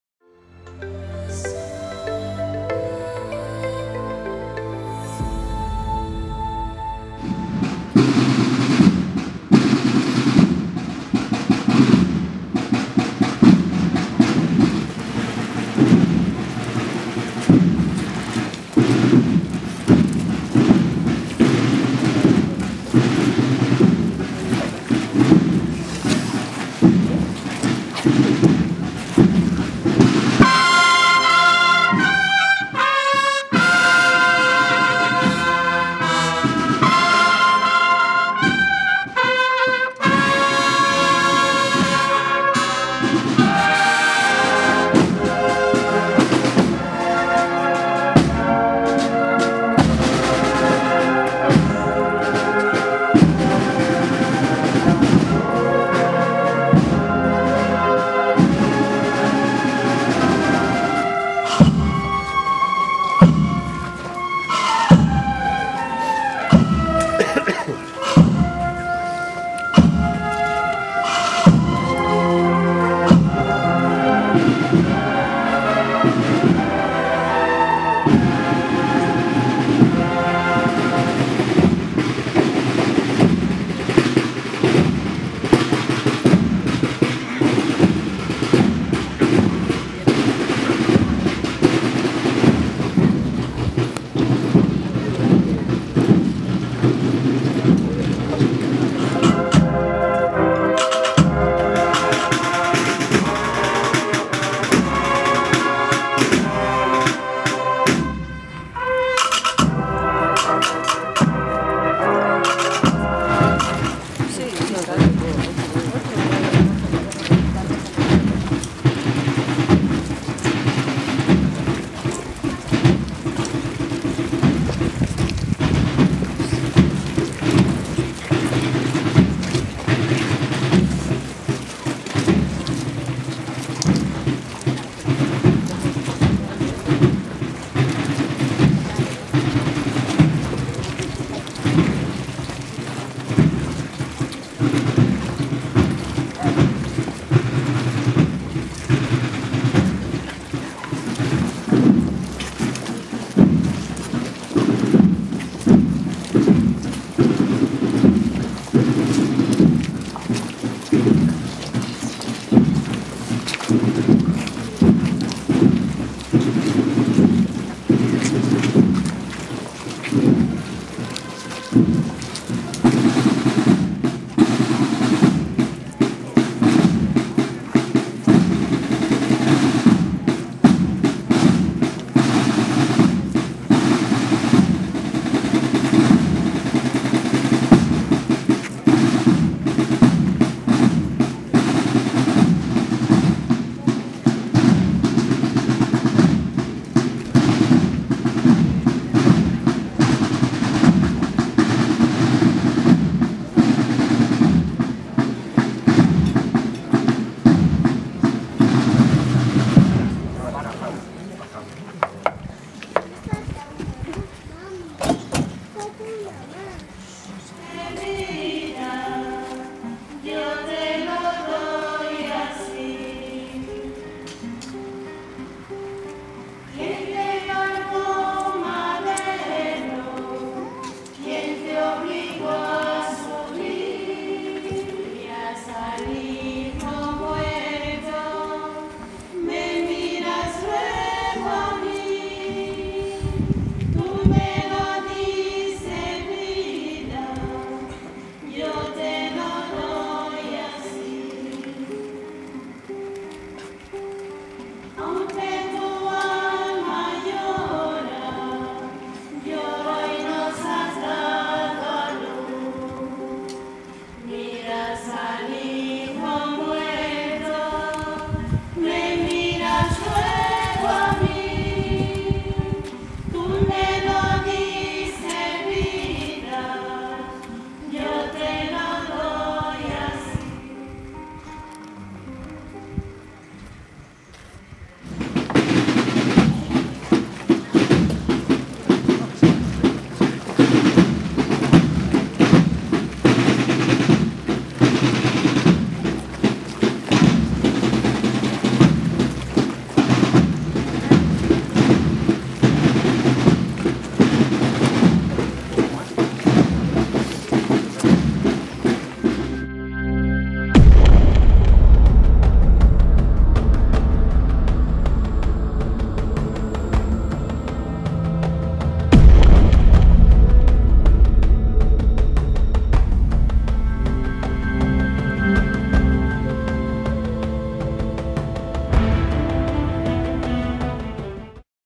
Procesión de la Soledad en Vitigudino 2014
Procesión de la Soledad en Vitigudino correspondiente a 2014 con las monjas de clausura dedicándole una canción a la Virgen en la plaza del Convento.